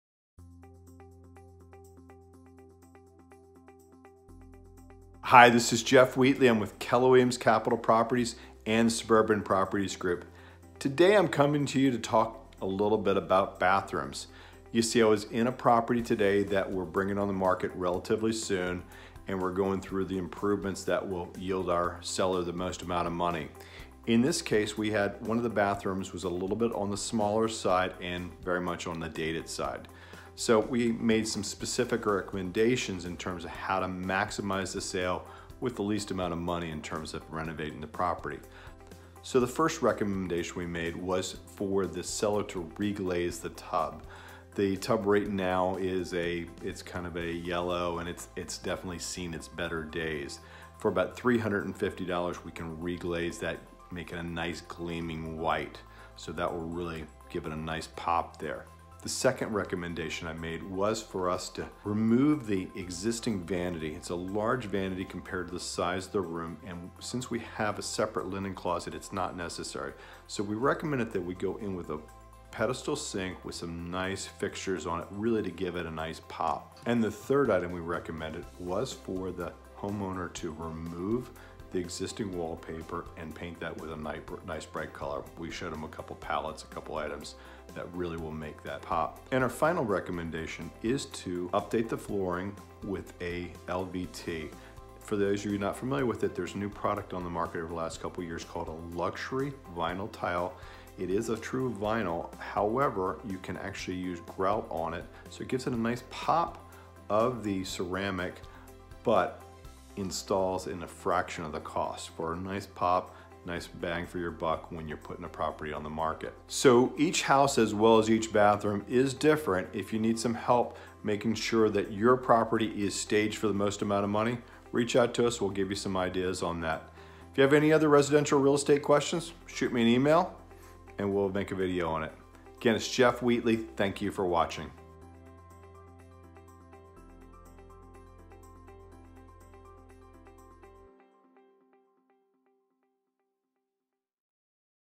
How We Help Our Clients Choose Upgrades
Upgrading your home before you list doesn’t have to involve a total rehaul. Today we’re highlighting four of the simple changes we recommended for one of our clients recently when it came time for them to fix up their bathroom. Bathroom remodels are some of the most lucrative in terms of return on value, but this doesn’t mean they should be pursued without careful planning. To learn how and why our team chose the upgrades we did for this client, watch this short video.

Jeff Wheatley
Jeff Wheatley Homes
Keller Williams Capital Properties
20 West Gude Drive #200 Rockville Md. 20850